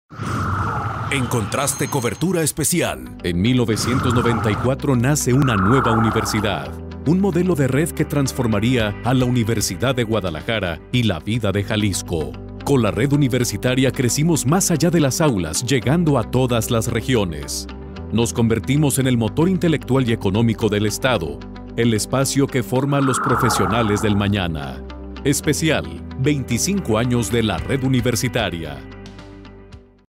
Promo || Especial 25 años de la Red Universitaria 14 noviembre 44
En 1994 nace una nueva universidad, un modelo de red que transformaría a la Universidad de Guadalajara y la vida de Jalisco.
Con la Red Universitaria crecimos más allá de las aulas llegando a todas las regiones. Nos convertimos en el motor intelectual y económico del estado; el espacio que forma a los profesionales del mañana.